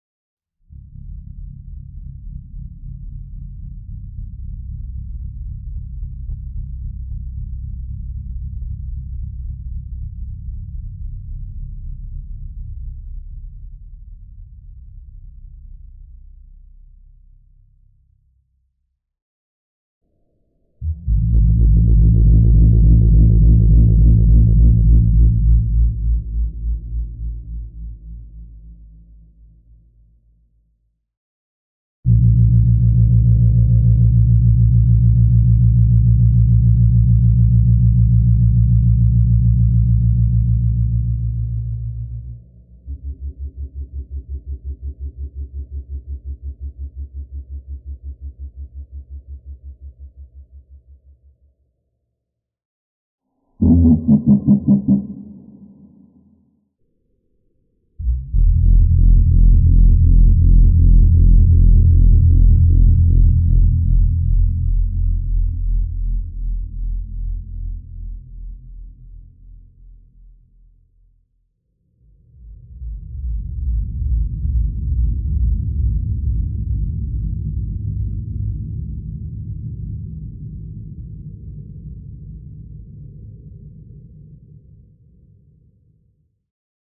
Giganotosaurus Sounds
Giganotosaurus was a very large theropod dinosaur, so I have some very large sounding low frequency rumbles for it. Hope you enjoy! (Use headphones)